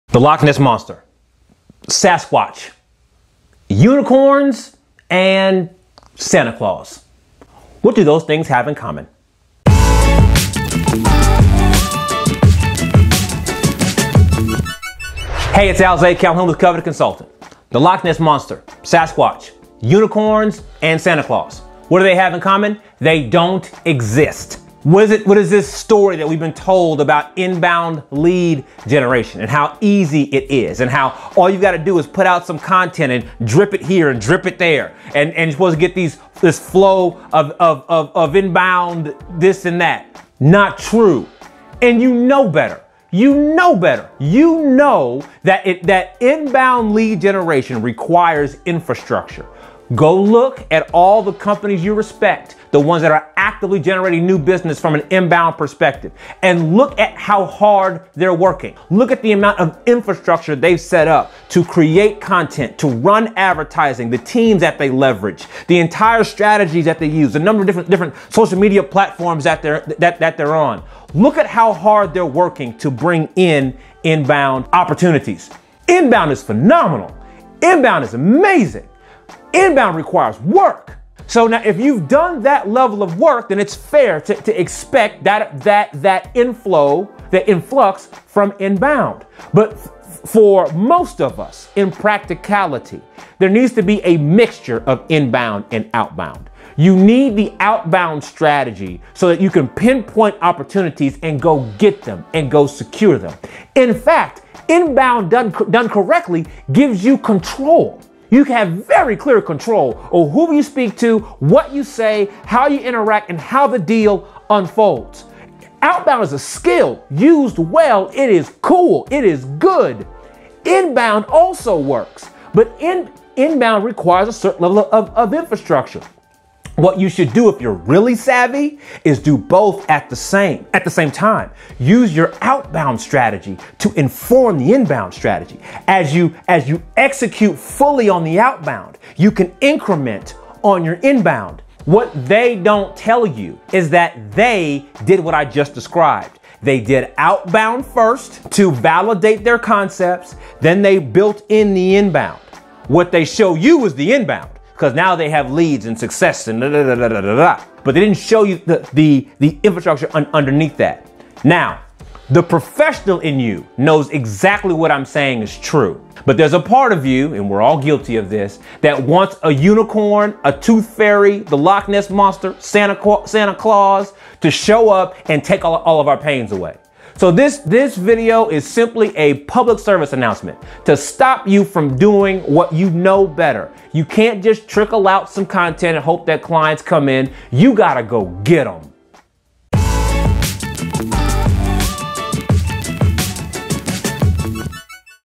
Inbound marketing lied to you... and you should know better
90 Day Video Challenge: 85 of 90

Isn't inbound lead gen supposed to be easy?

You should know better.

Everything requires work and strategy, including inbound lead generation.